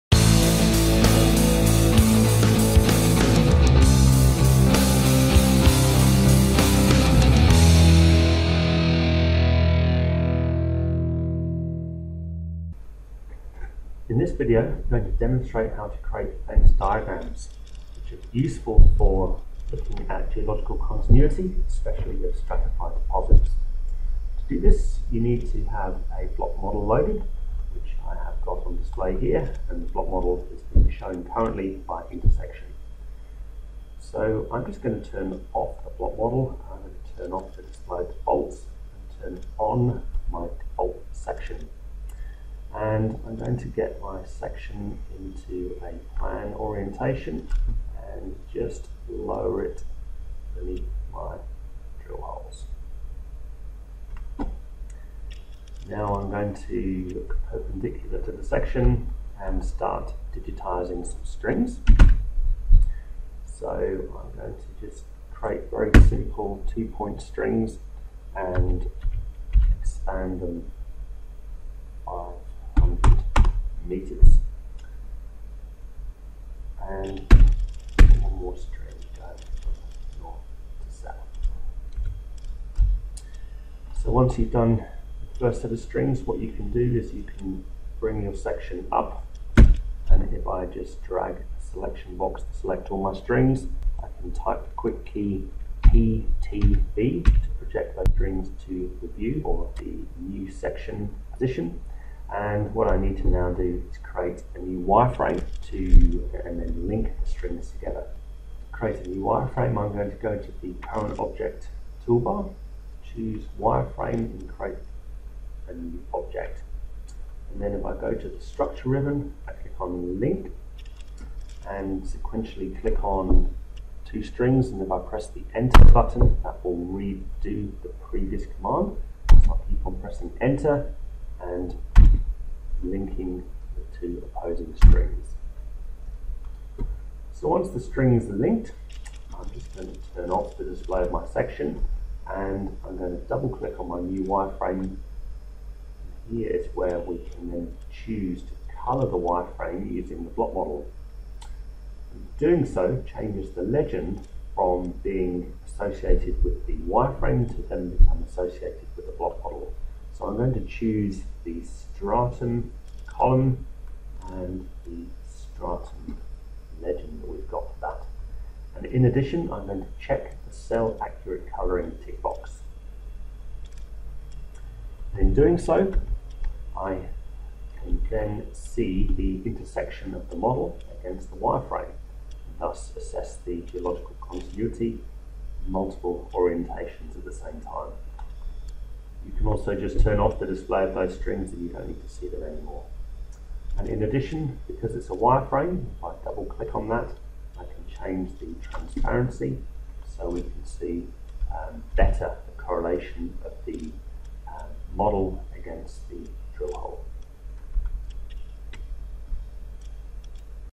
Datamine Studio RM - Fence Diagrams
How to create 3D fence diagrams to view geological continuity of the block model from multiple orientations.